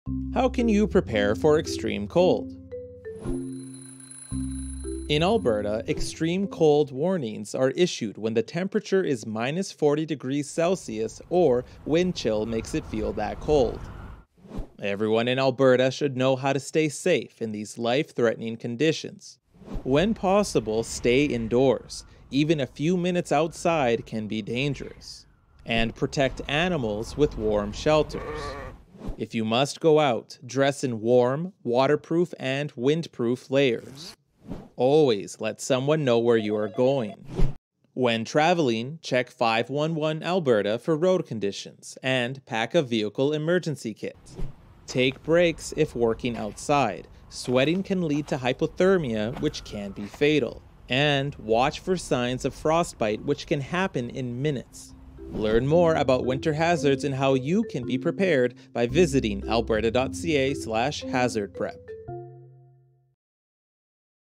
How can you prepare for Extreme Cold
Learn how to stay safe when the temperature drops below -40 degrees Celsius or wind chill makes it feel that cold. to learn how to be prepared for Extreme Cold.